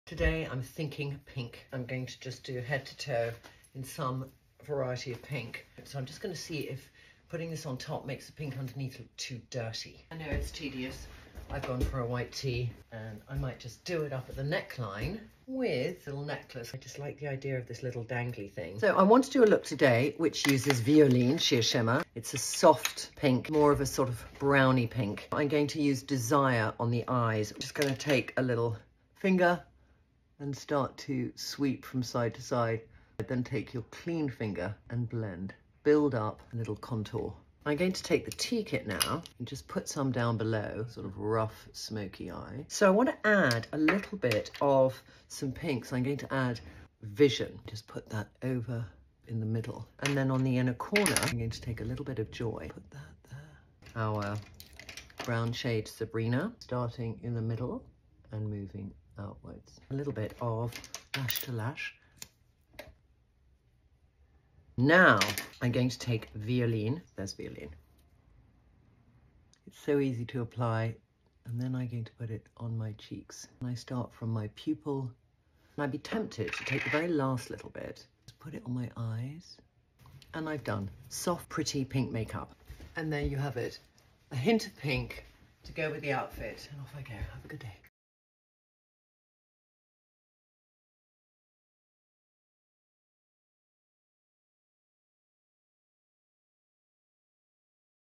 OOTD: Pretty In Pink With Our Latest Sheer Shimmer Shade | Trinny
Trinny’s thinking pink today in homage to the latest addition to Trinny London’s lip and cheek lineup – Sheer Shimmer in Violine, a softly feminine, natural rosebud shimmer that brings life to the complexion and looks good on absolutely everyone. There’s a time and a place for a full-blown fuchsia moment, but there’s nothing like a fresh rosy glow to make you feel pretty in pink…

Sheer Shimmer in Violine
Eye2Eye in Desire
Eye2Eye in Vision
Eye2Eye in Joy
T-Kit Brush
Line2Define in Sabrina
Lash2Lash Mascara
Co-ord: ME+EM
T-shirt: Zara